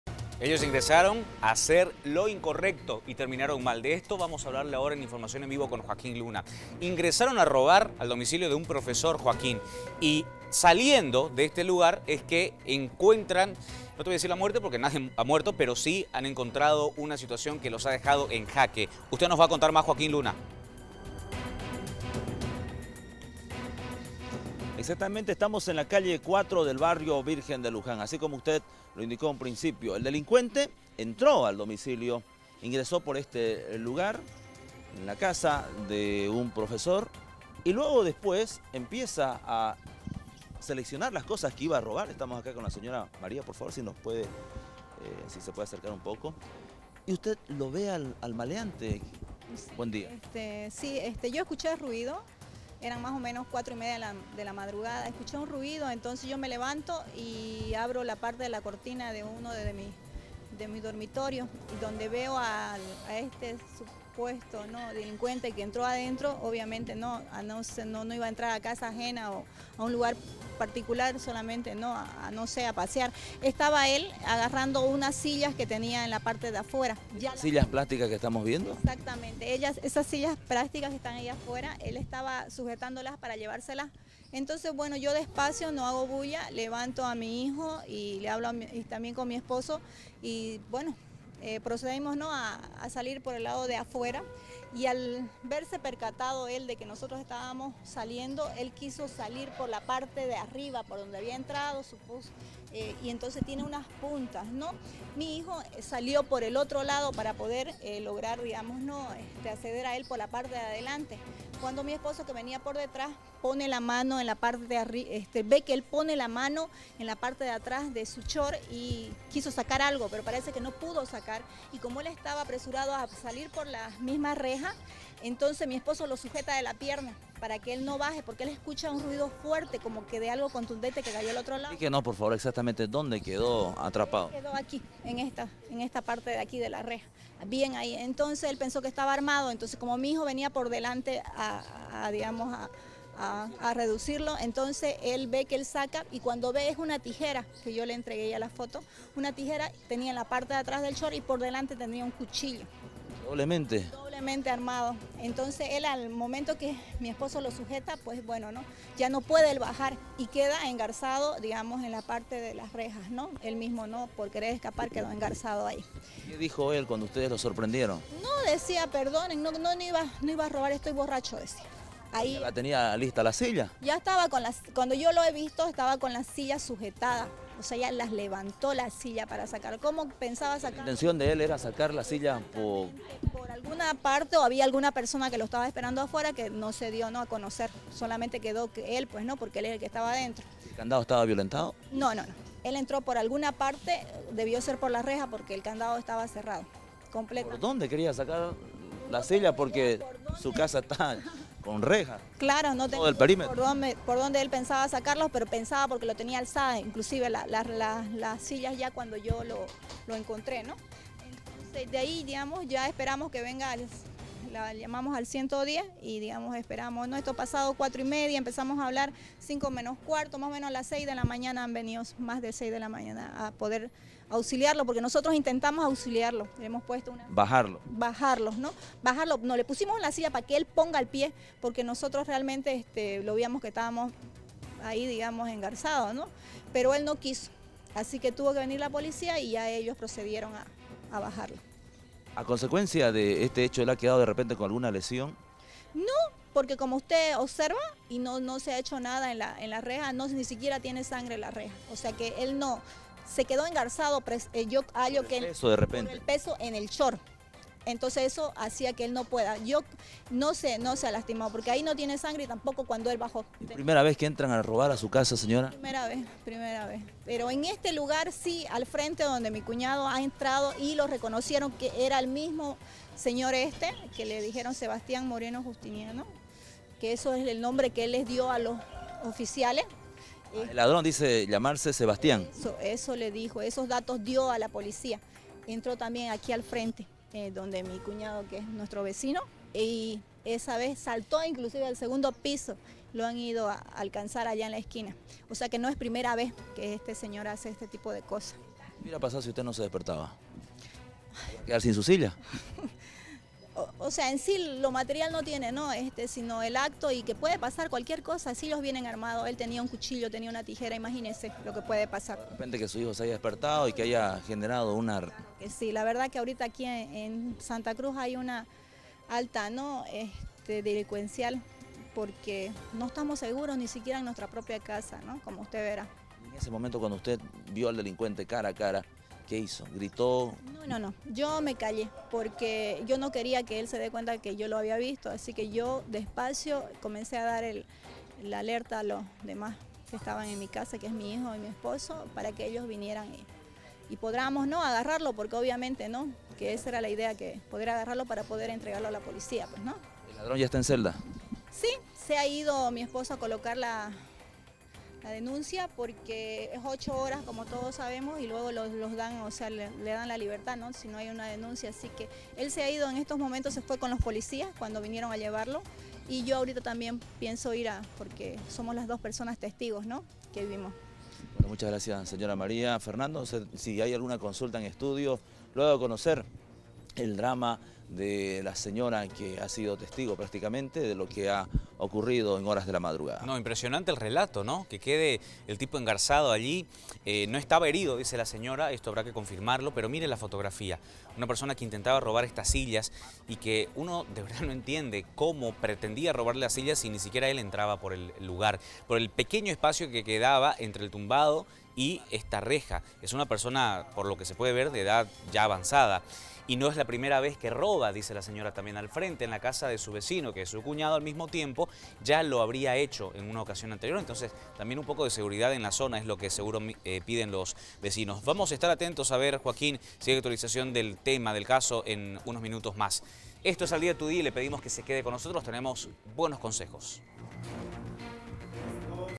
Ladrón queda atrapado entre fierros
Ingresó a robar a una casa y no pudo salir.